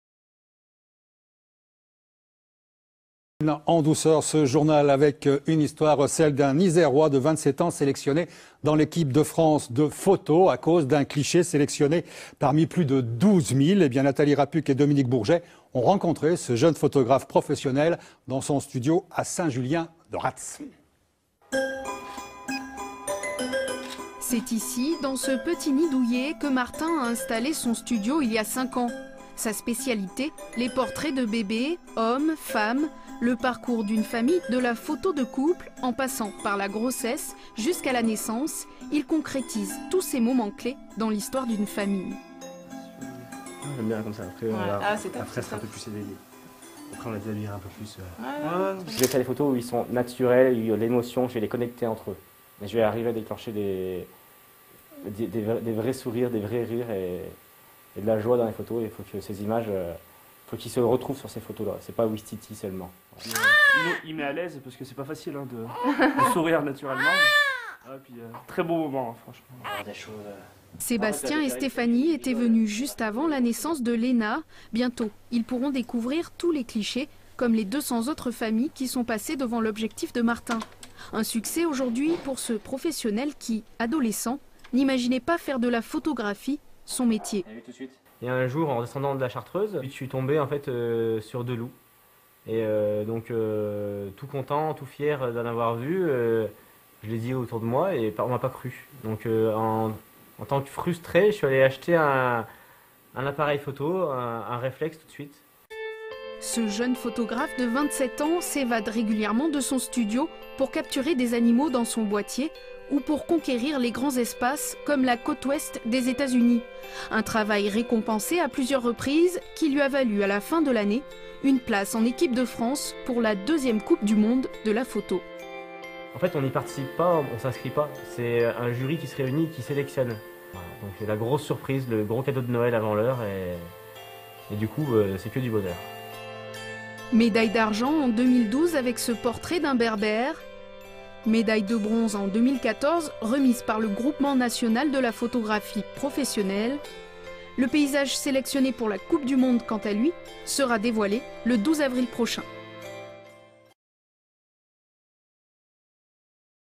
Reportage France 3 au Studio Martin Morel pour la Coupe du Monde
Suite à la sélection du Studio Martin Morel en équipe de France pour la Coupe du Monde la Photographie 2015, une équipe TV de France 3 est venue au Studio réaliser un reportage.